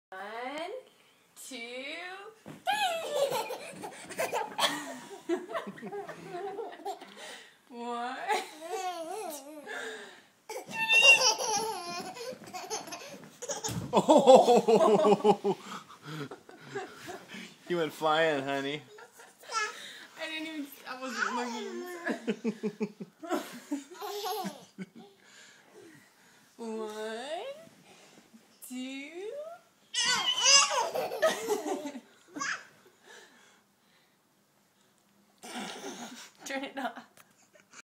Abby bounce
Abby on the air mattress in guest room